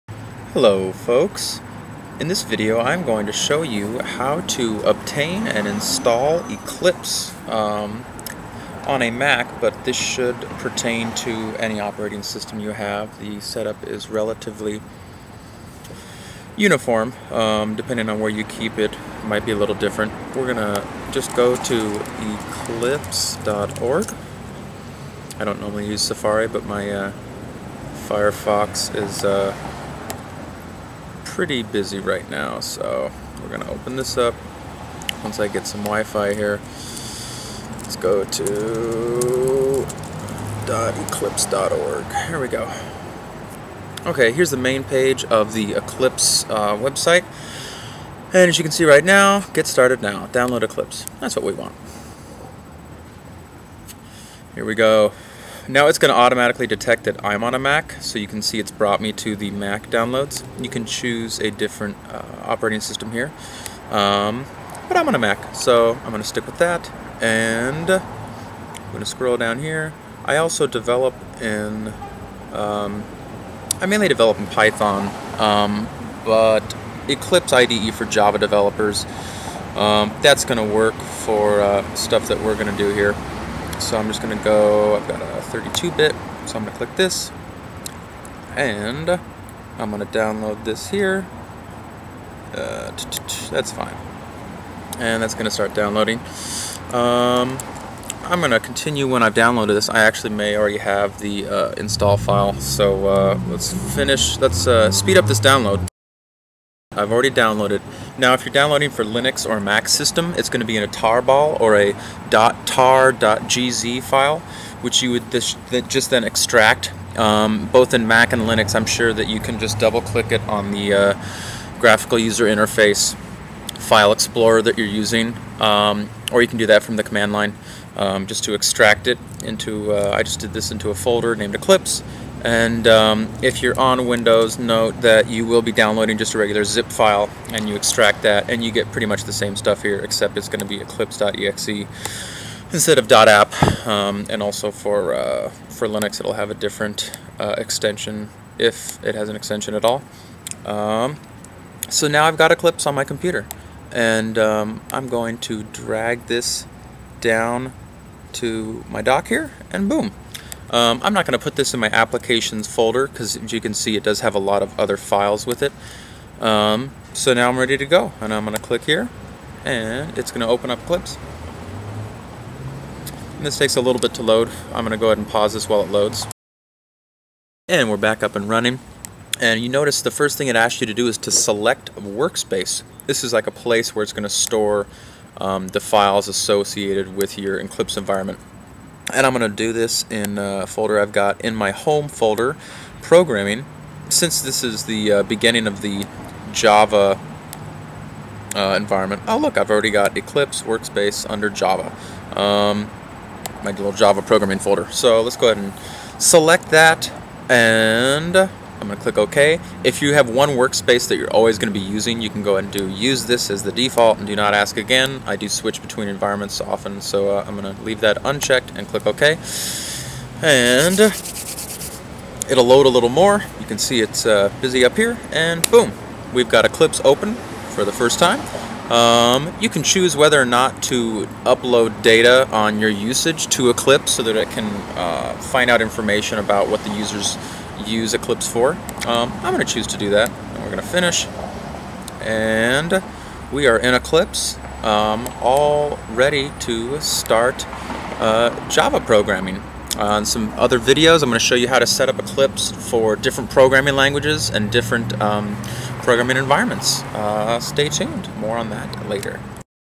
SalesForce.com: Download and Install Eclipse
Let's get programming!  We're going to need Eclipse to install the Force.com IDE plugin for it, so let's get it!  I'm on a Mac, but this video should walk you through any operating system install.  It's pretty similar.